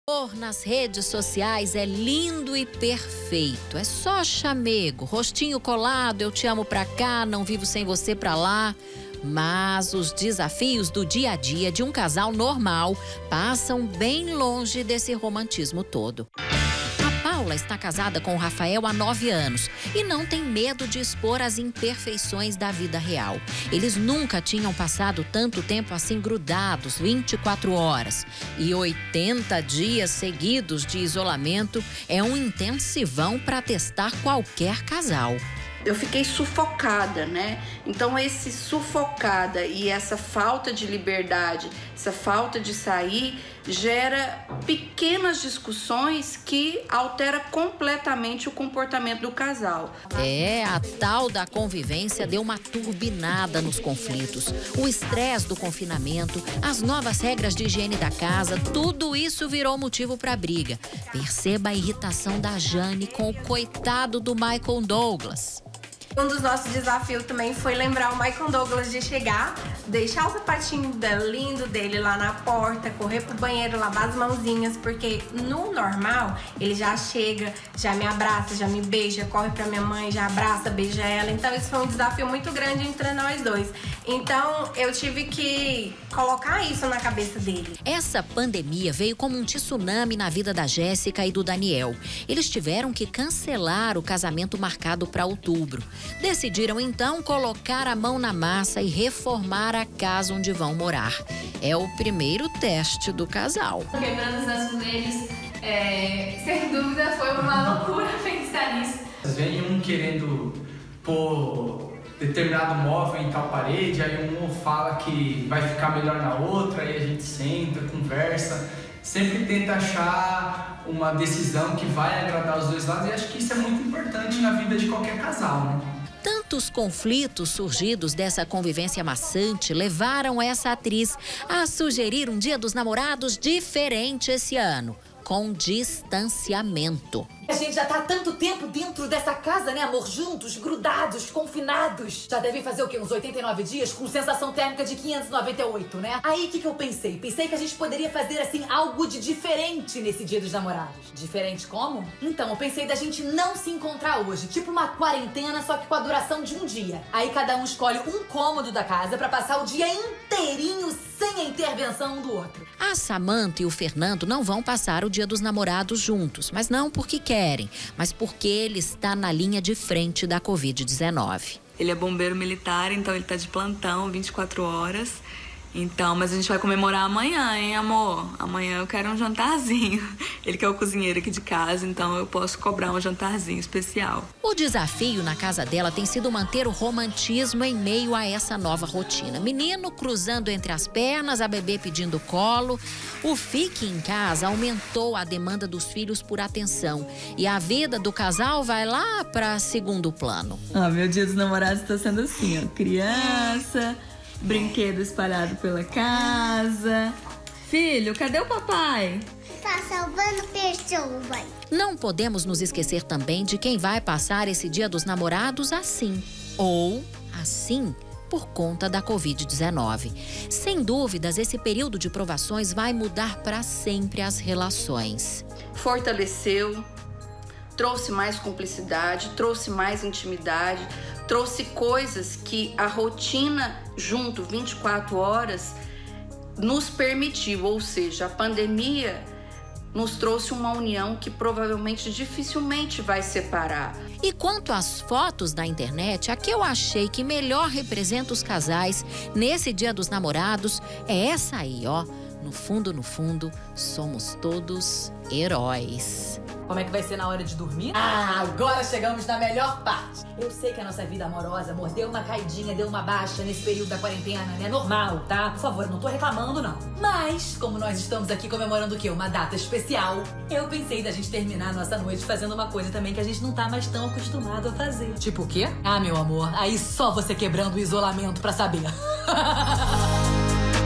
DIAS DOS NAMORADOS DESAFIOS PARA MANTER RELACIONAMENTO NA PANDEMIA
Hoje, dia dos namorados , a internet estava cheia de fotos românticas. Mas todo mundo sabe que nem tudo são flores na relação a dois. Com o isolamento social, então, os casais são obrigados a ficar 24 horas juntos durante os 7 dias da semana. Os desafios, claro, multiplicaram mas, nada que o amor não supere, não é mesmo?